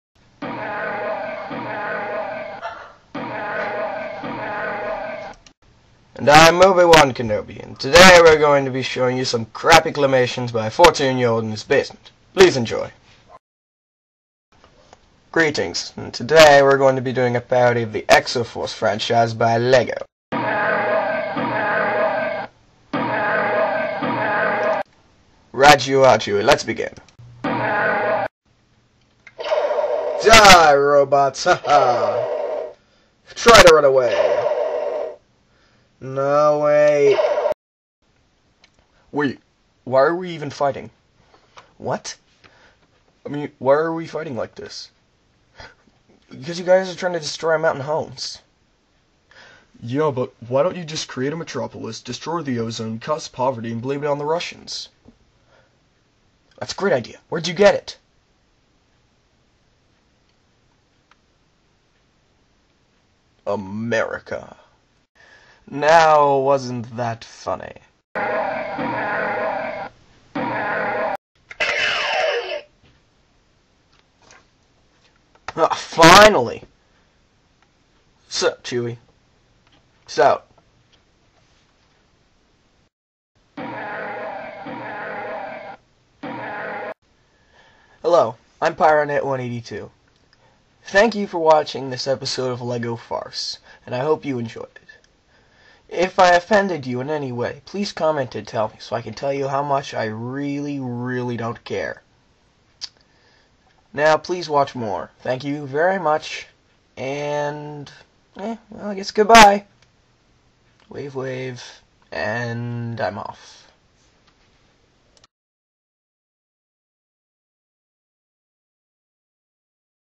Lego Farce-Exo Farce
This is my first of (hopefully) many videos making fun of lego and all things it enjoys. Later, I might also team up with Gaamari Productions in order to make a SUPER VIDEO!

Your's truly,
                     pyronet182